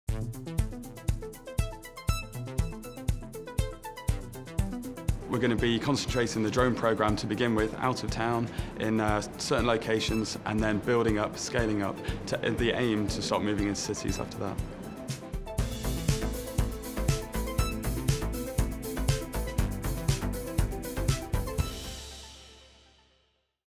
CoStar: Innovation. Deploying Drones to Photograph Buildings
CoStar uses drones to photograph buildings, enhancing the data around CRE listings.